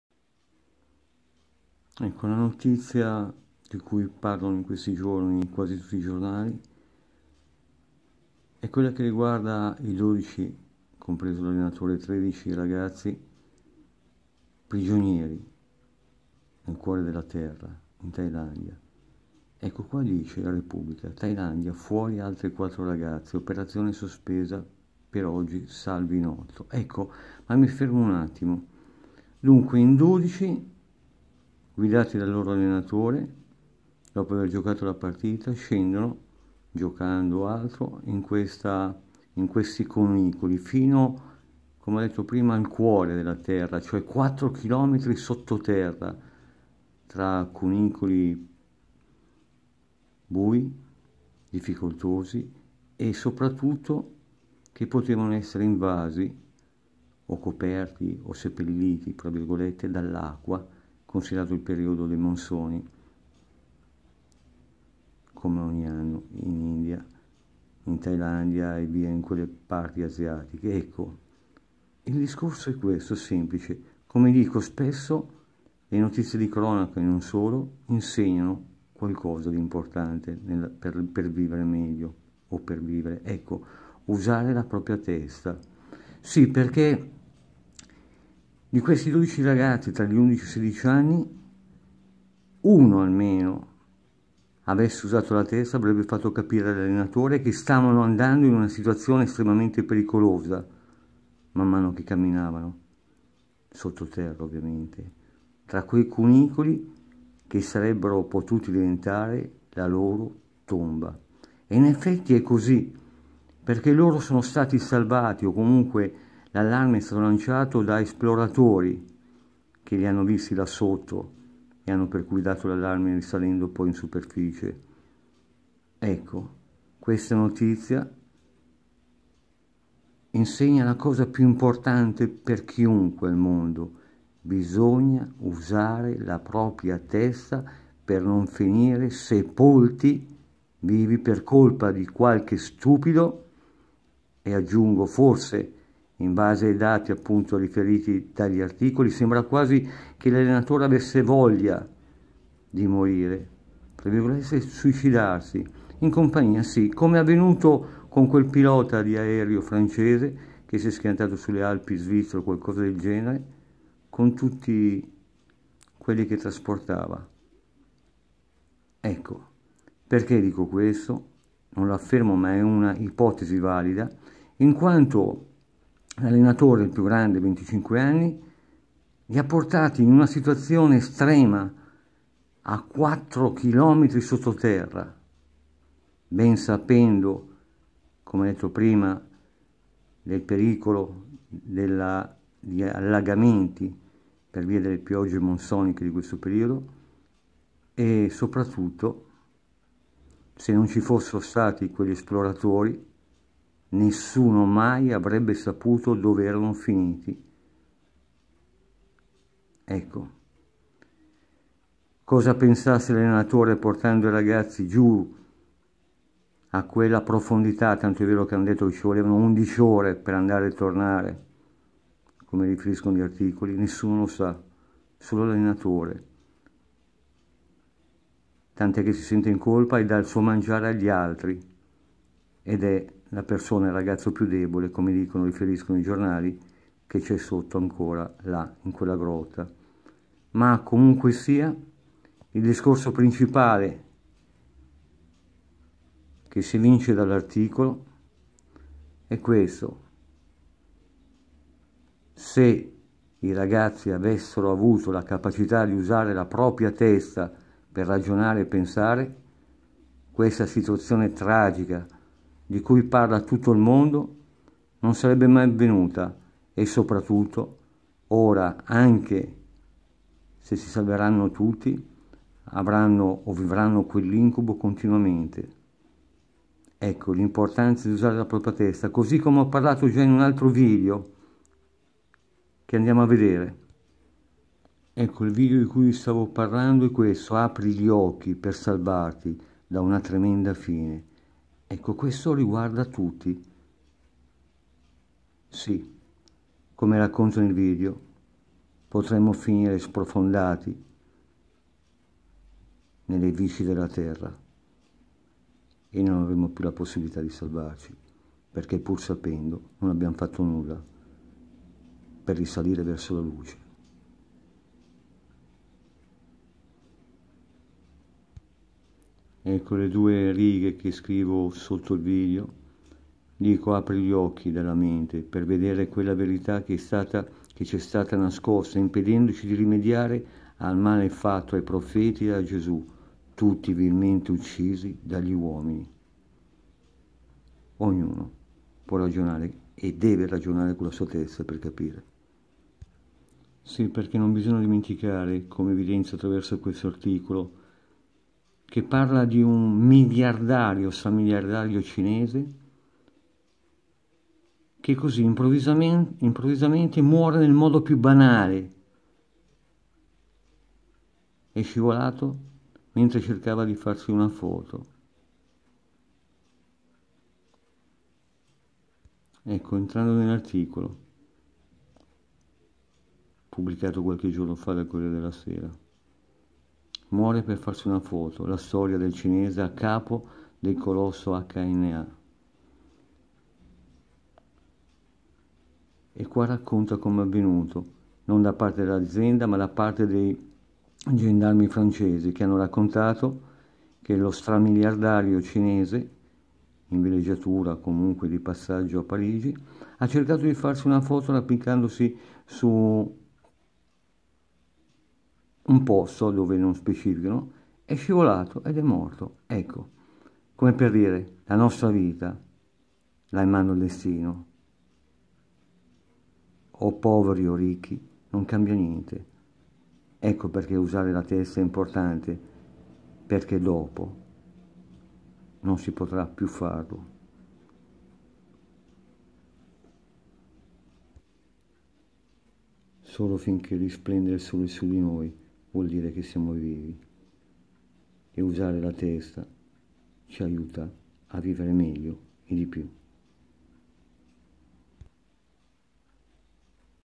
Tailandia chi non usa la Testa finisce vivo sotto terra!
Se almeno uno dei 13 ragazzi avesse usato la propria testa avrebbe impedito al loro caro allenatore di portarli a tale profondità sotto terra dove sarebbero "spariti" per sempre!

Vedere video:
"Apri gli occhi per "Salvarti" da una tremenda Fine!"